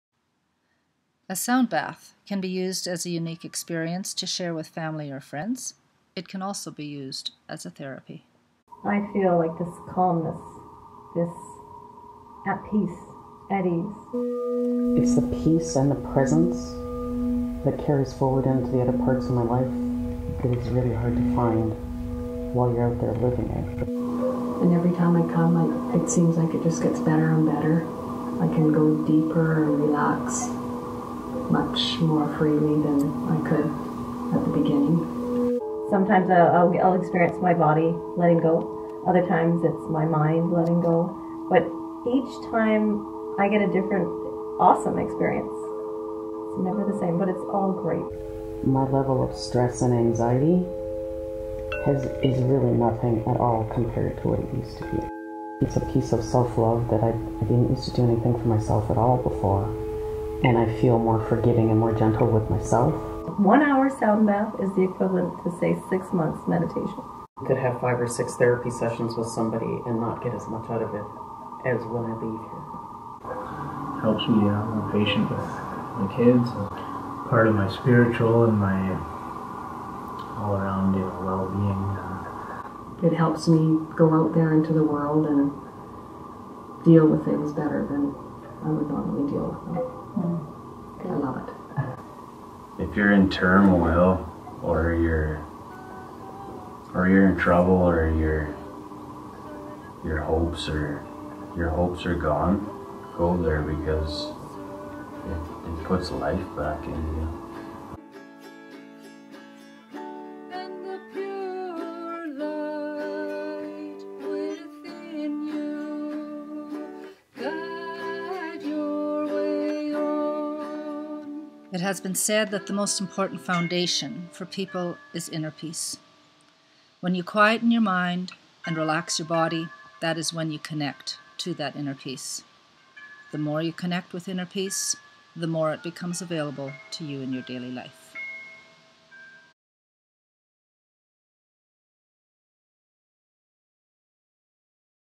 Ground with Sound-Why Have A Soundbath
Feedback and thoughts from "Soundbathers".   Ground with Sound is located in Grande Prairie, AB. Canada